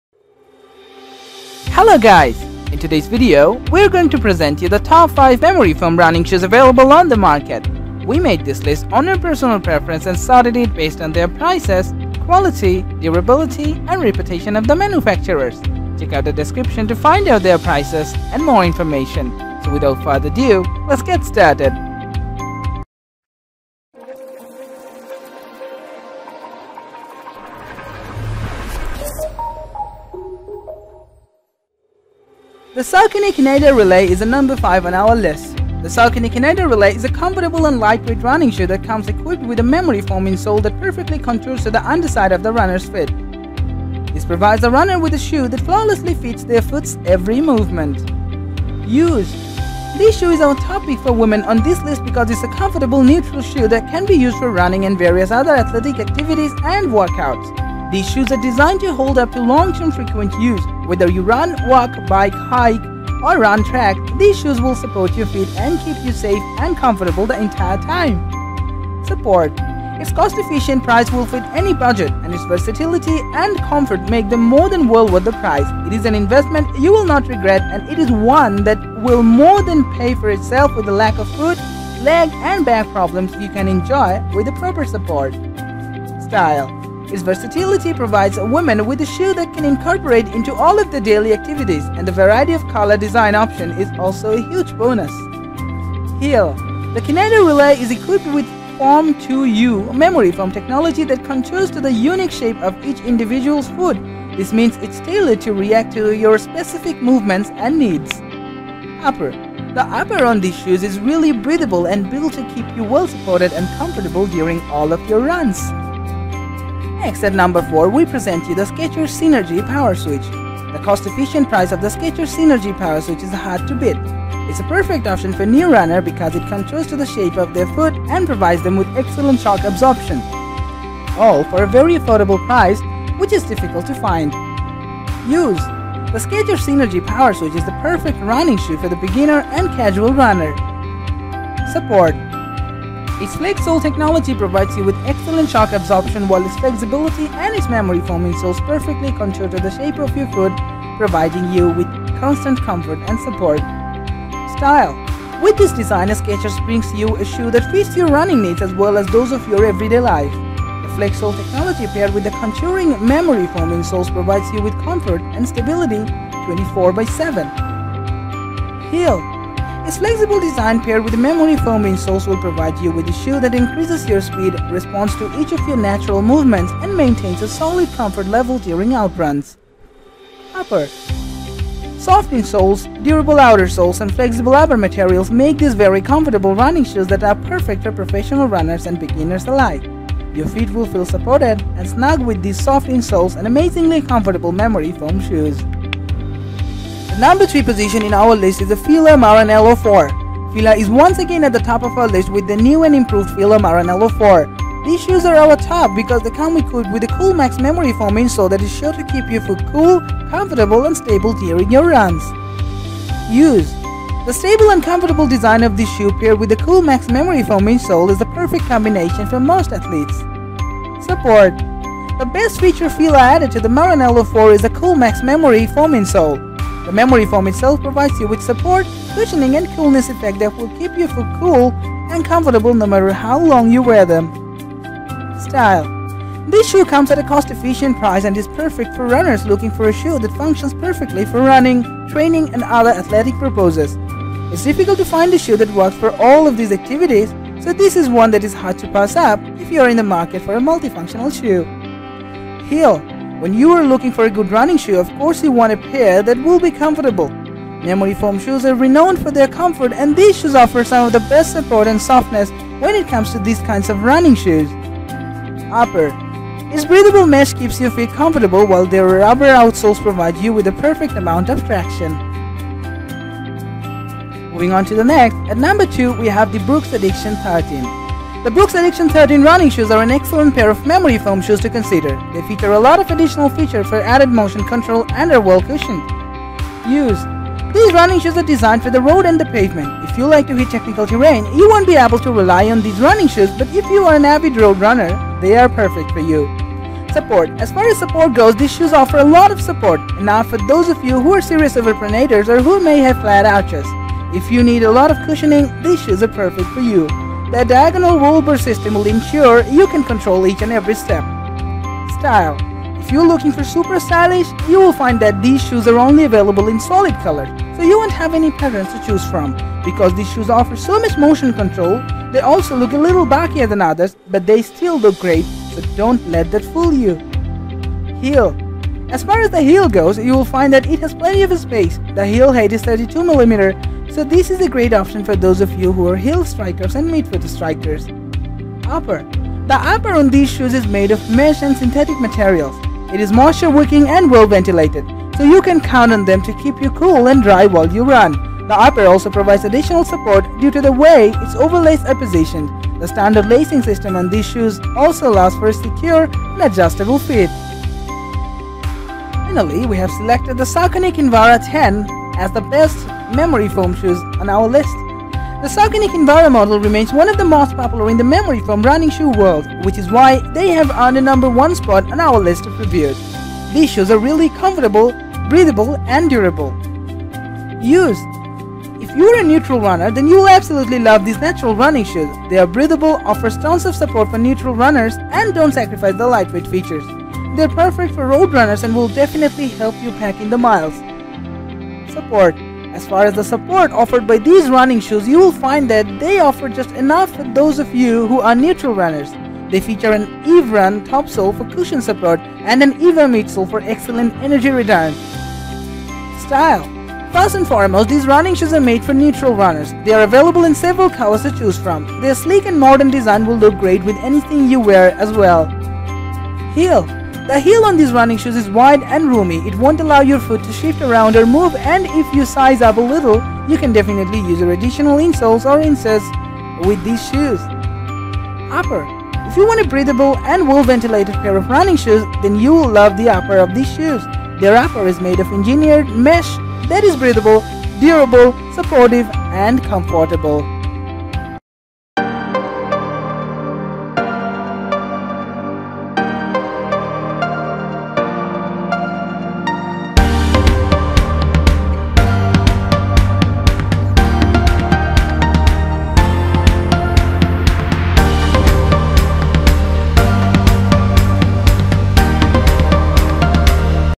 Best Memory Foam Running Shoes Reviews in 2024 | Best Budget Memory Foam Running Shoes
Are you searching for the Memory Foam Running Shoes of 2024? These are the Top 5 Memory Foam Running Shoes we found so far:

5. Saucony Kineta Relay [Amazon]
4. Skechers Synergy Power Switch [Amazon]
3. Fila Maranello 4 [Amazon]
2. Brooks Addiction 13 [Amazon]
1. Saucony Kinvara 10 [Amazon]

To narrow down your searching effort, we have researched the market on the Memory Foam Running Shoes. We already spent hours analyzing these top 5 Memory Foam Running Shoes to ensure your worth buying.They all come with excellent features with a great price range.

This video on the Memory Foam Running Shoes reviews in 2024 will add value for the money. So keep watching till the end and select the suited one for you.

Visit Review Infinite Top Videos